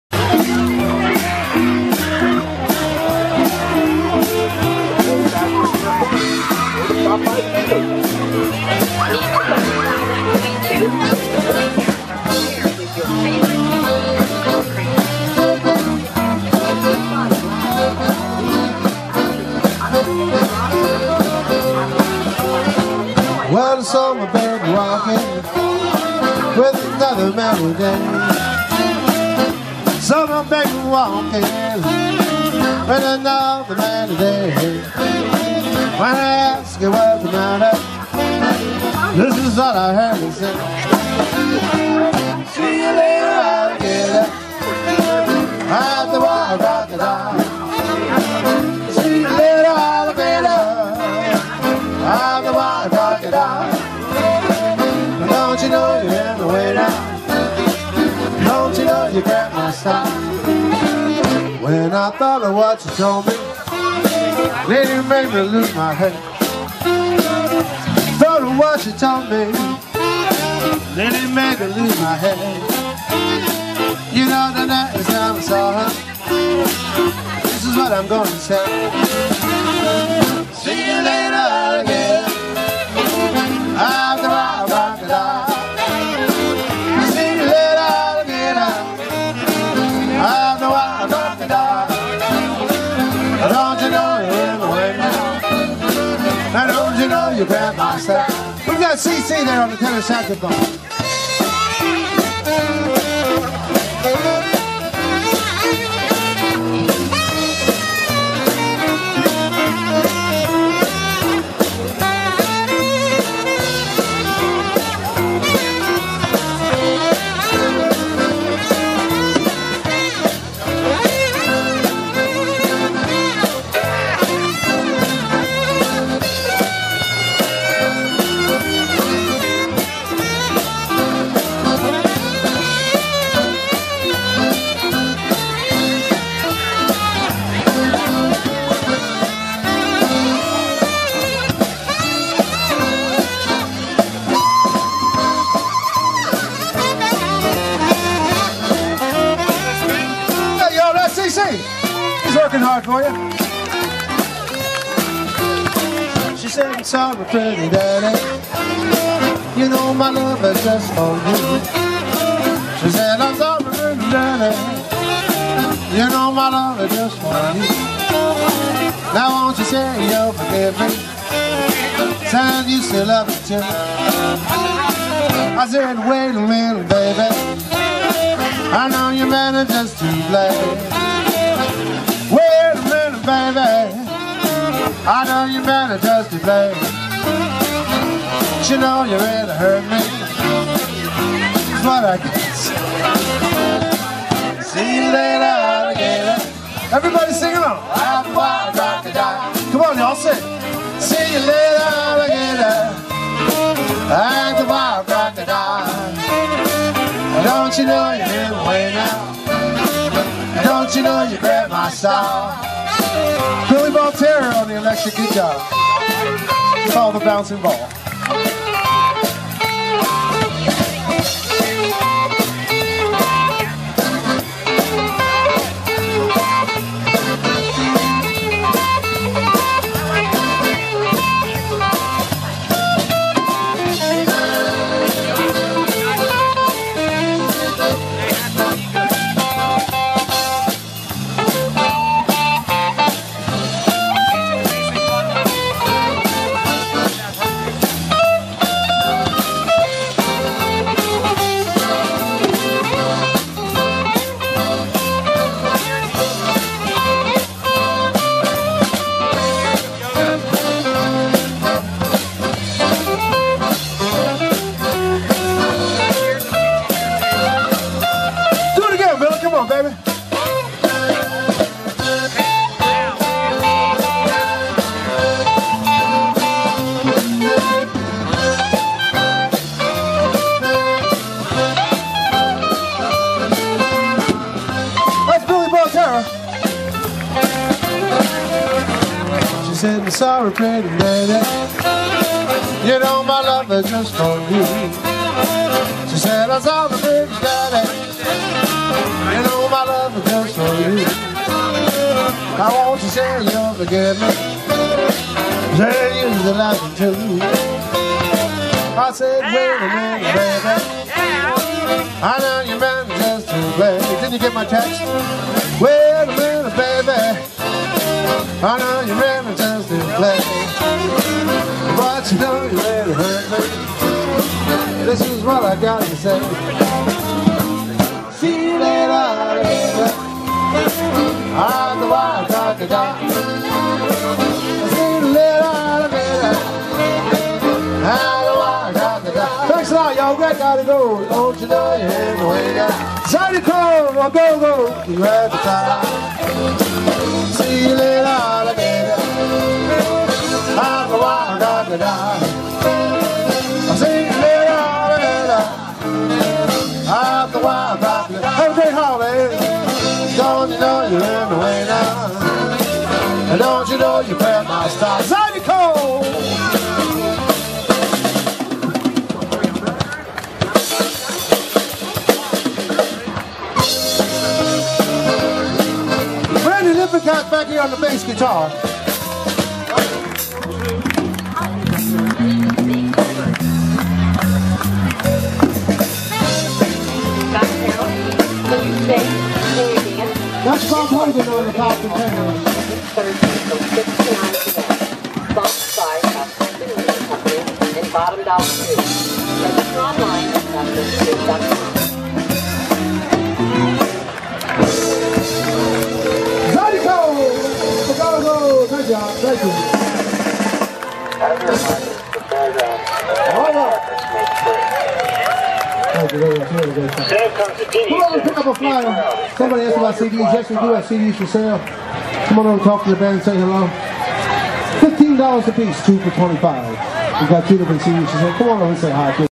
zydeco a gogo
Caught these guys playing at the fair.